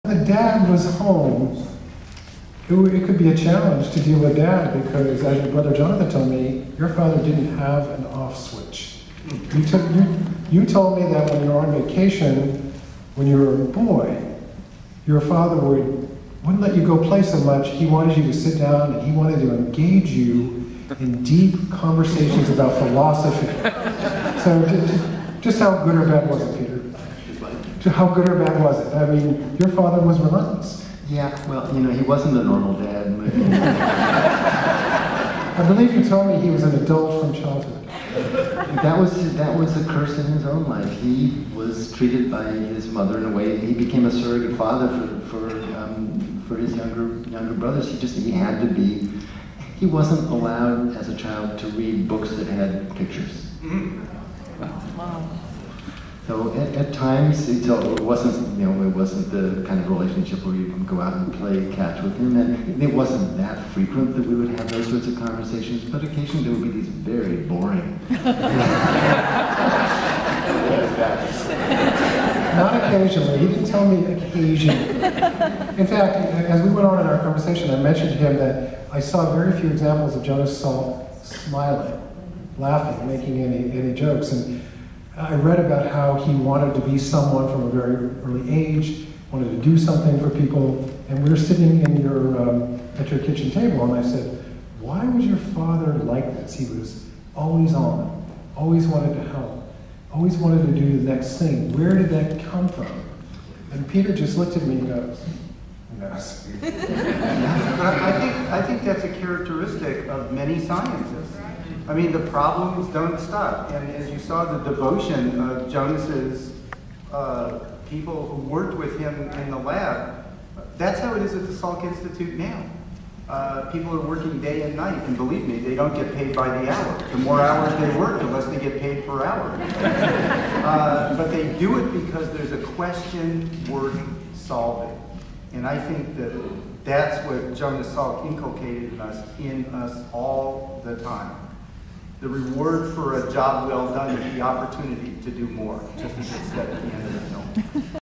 No off switch for Jonas Salk
Peter Salk, a son of Jonas Salk, says his father was always thinking and encouraged him to think. Taken Friday, Nov. 7, 2014, at a U-T San Diego event honoring the legacy of Jonas Salk, held in La Jolla at the Museum of Contemporary Art San Diego.